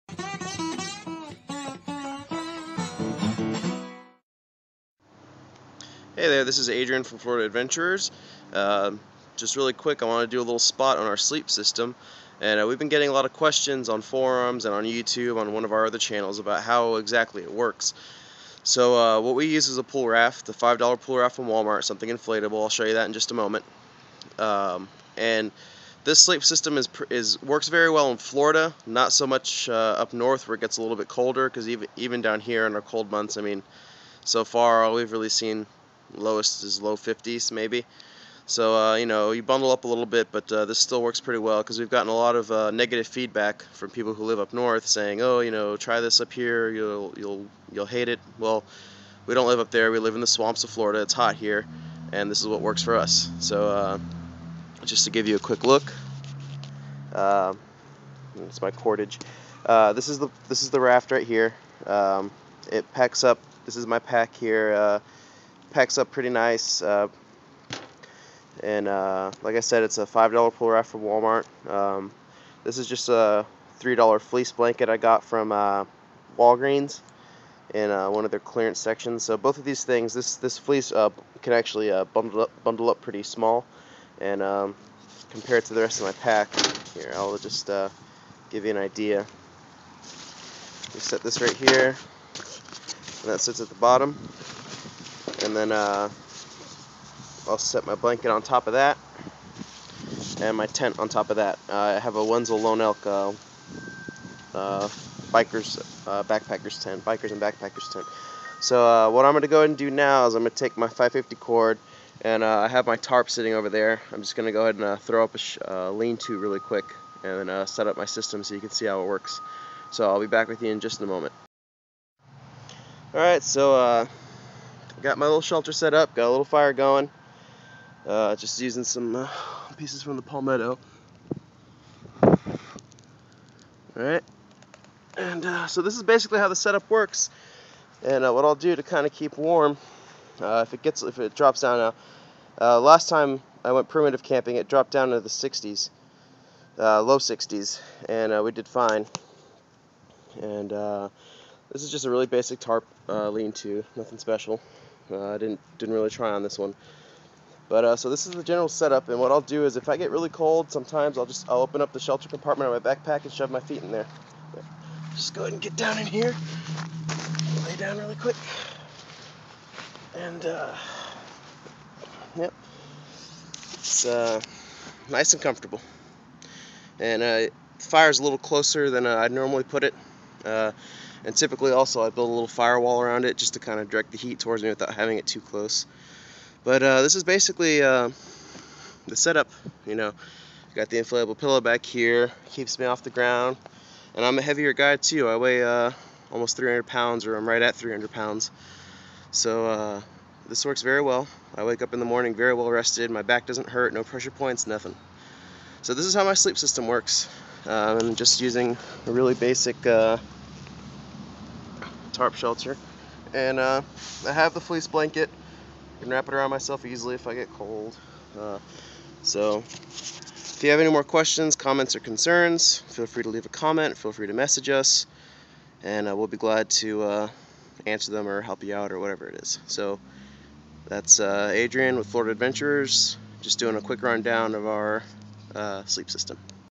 Pool raft sleep system and set up
We have gotten a lot of questions and feedback concerning our sleep system. So here it is, in action. This is a system that works for us in Florida, it may not preform so well in colder states. We hope you found this informative.